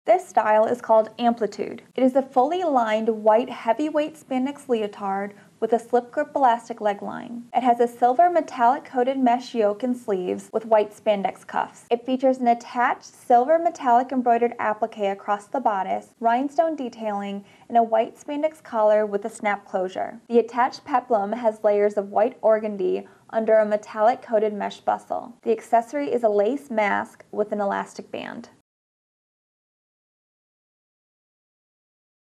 Amplitude | TH21007 | Tenth House Costume Collection
Heavyweight spandex leotard is fully-lined with spandex and has a slip grip elastic leg line, metallic coated mesh yoke, and sleeves with spandex cuffs. Features attached metallic embroidered appliqué, rhinestone detailing, and spandex collar with snap closure. Attached peplum has layers of organdy under a metallic coated mesh bustle.

Includes: lace mask, hanger, and garment bag